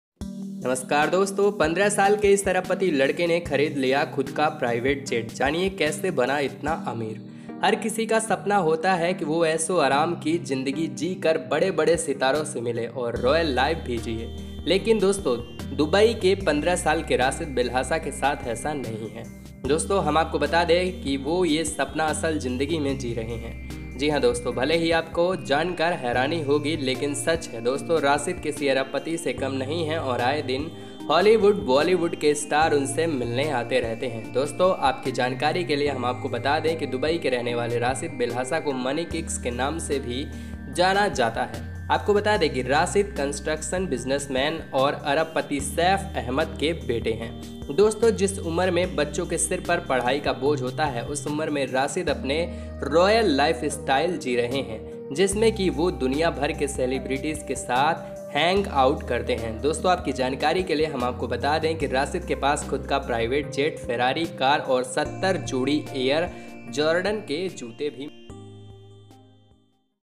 15 साल के इस अरबपति लड़के ने खरीद लिया खुद का प्राइवेट जेट, जानिए कैसे बना इतना अमीर !
हर किसी का सपना होता है कि वह ऐसो आराम की जिंदगी जी बड़े-बड़े सितारों से मिले और रॉयल लाइफ भी जिए, लेकिन दोस्तों दुबई के 15 साल के राशिद बेल्हासा के साथ ऐसा नहीं है.दोस्तों हम आपको बता दे कि वह यह सपना असल जिंदगी में जी रहे हैं. जी हां दोस्तों भले ही आपको जानकर विश्वास नहीं हो रहा होगा लेकिन सच है. दोस्तों राशिद किसी अरबपति से कम नहीं है और आए दिन हॉलीवुड बॉलीवुड स्टार उनसे मिलने आते रहते हैं.दोस्तों आपकी जानकारी के लिए हम आपको बता दें कि दुबई में रहने वाले राशिद बेल्हासा को मनी किक्स के नाम से भी जाना जाता है. आपको बता दें कि राशिद कंस्ट्रक्शन बिजनेसमैन और अरबपति सैफ अहमद के बेटे हैं.दोस्तों जिस उम्र में बच्चों के सिर पर पढ़ाई का बोझ होता है, उस उम्र में राशिद अपने रॉयल लाइफ स्टाइल जी रहे हैं जिसमें कि वह दुनियाभर के सेलेब्रिटीज के साथ हैंग आउट करते हैं. दोस्तो आपकी जानकारी के लिए हम आपको बता दें कि राशिद के पास खुद का प्राइवेट जेट, फेरारी कार है और 70 जोड़ी एयर जॉर्डन के जूते भी हैं.